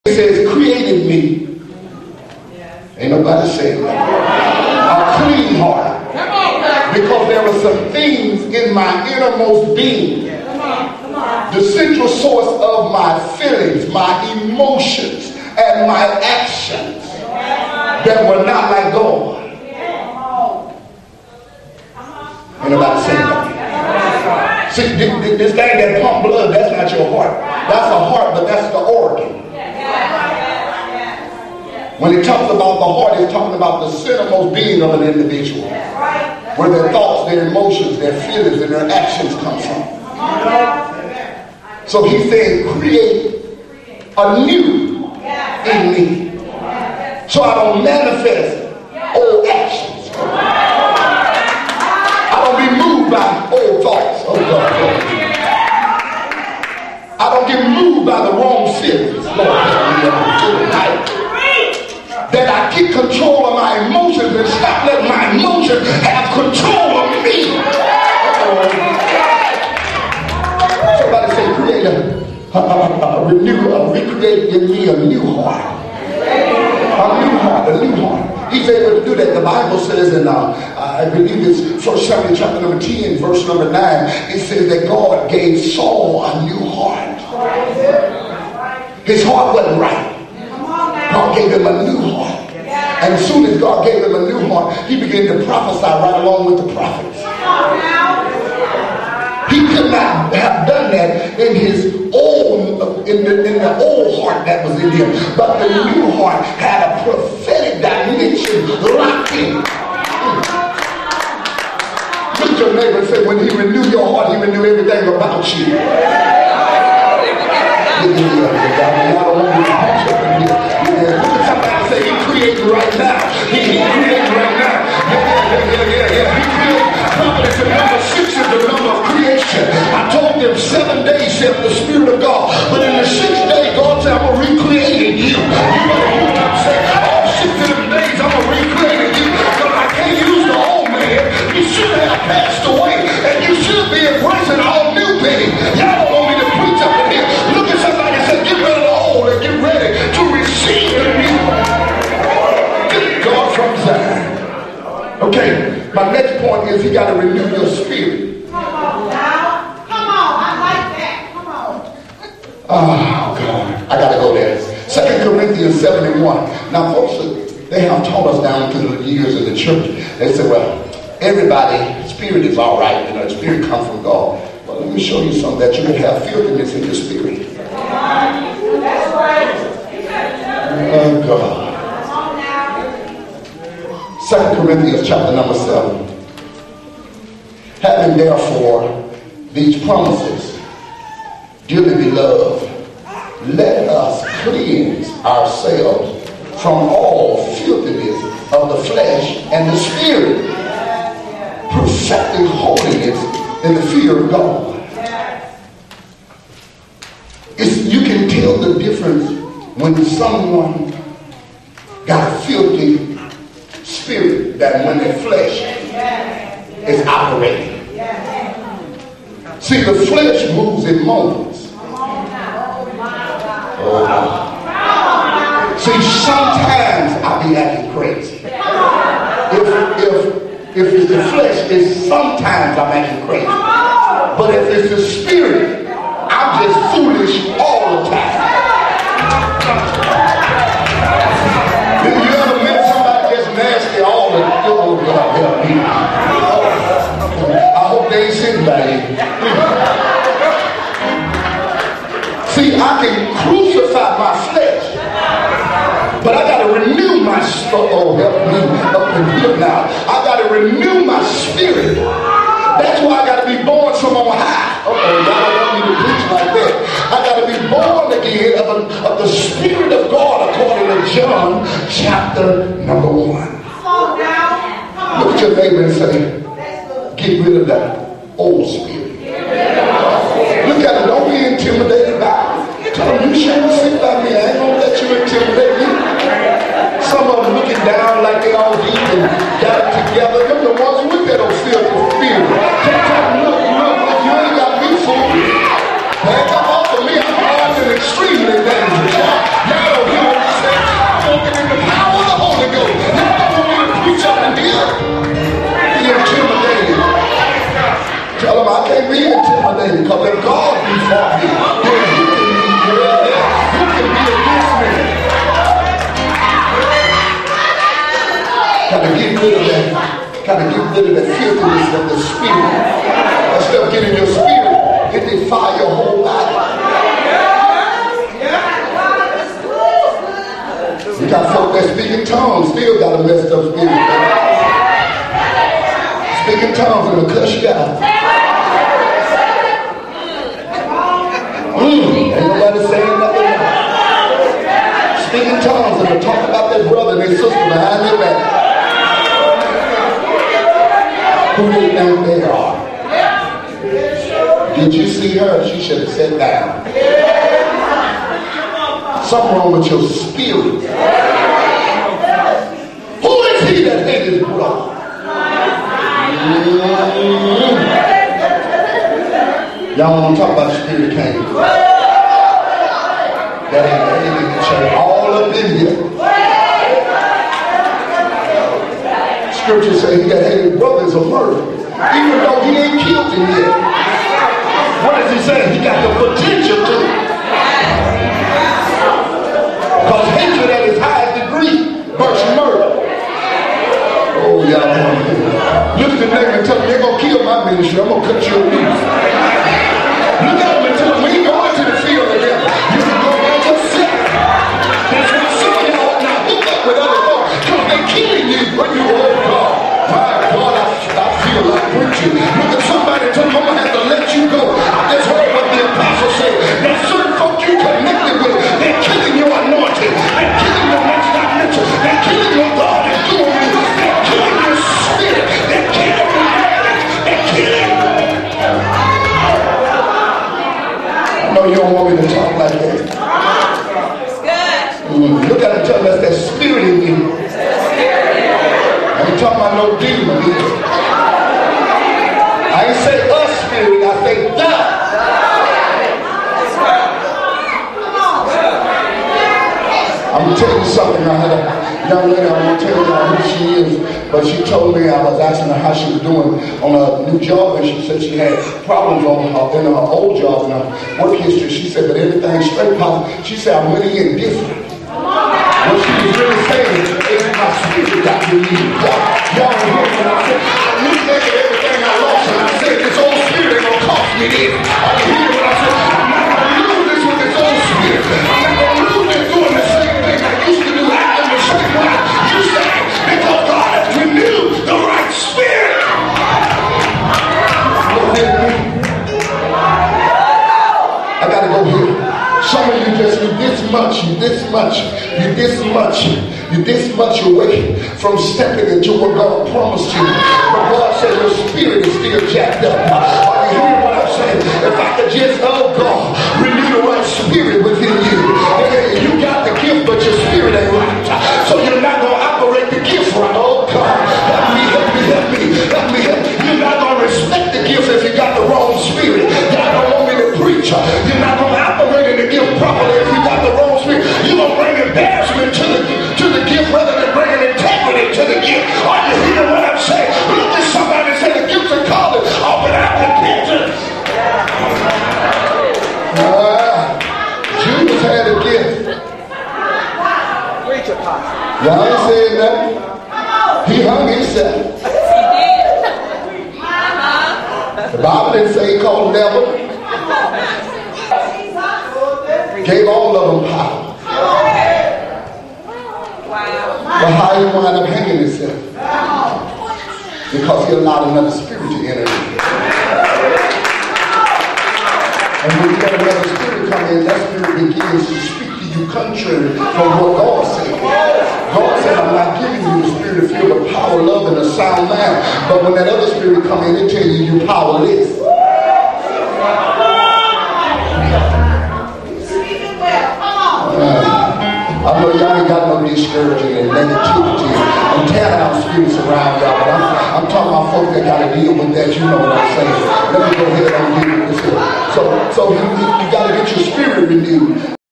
God's Man and His Mess Part 5.flv
Apostle Jones ministers at SAKAL Global Ministries' Men Conference concerning how you can be a man of God, but still have some area in your life you are struggling with.